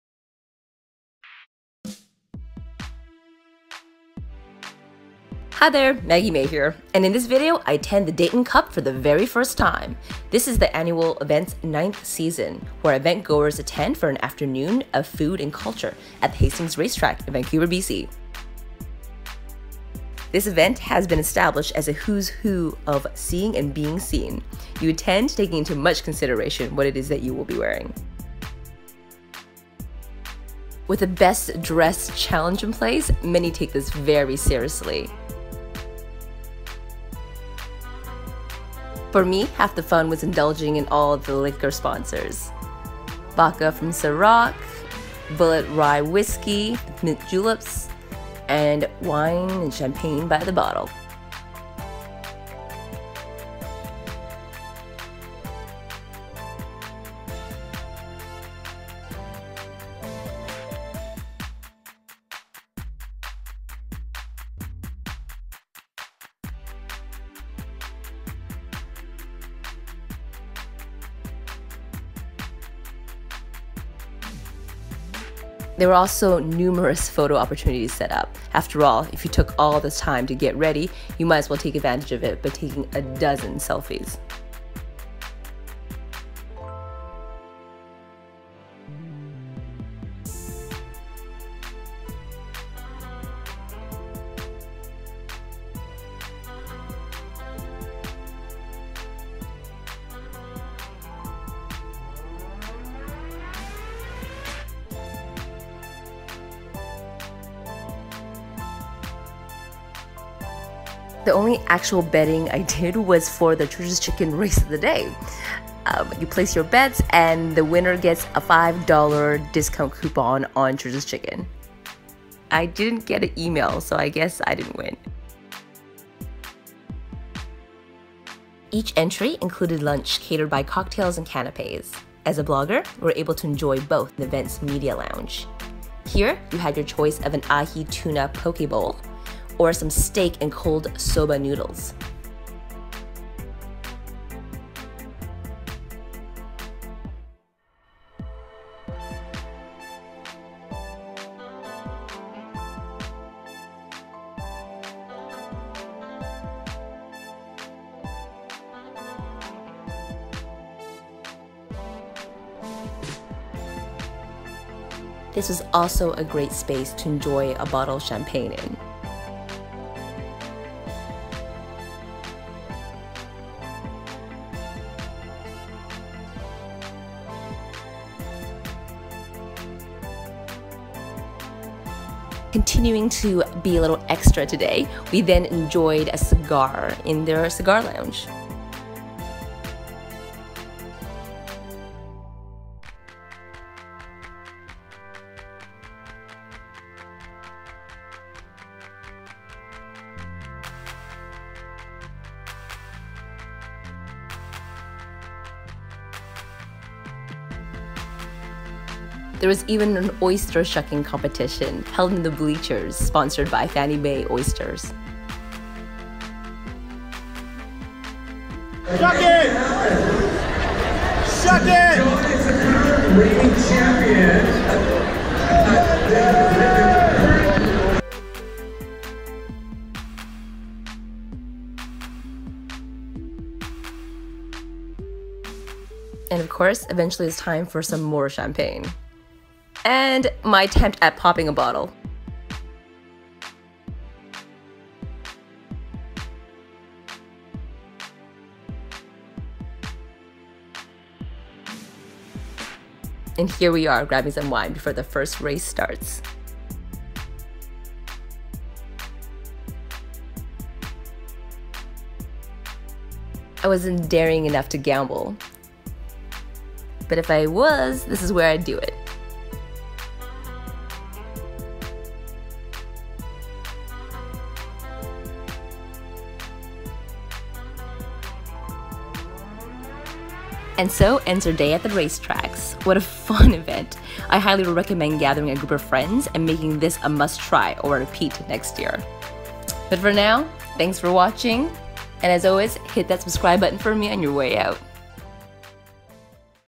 Champagne, cigars, and big floppy hats at the Deighton Cup
I attend this season's IT event, combining high fashion with large hats, cocktails, cigars, and horse races.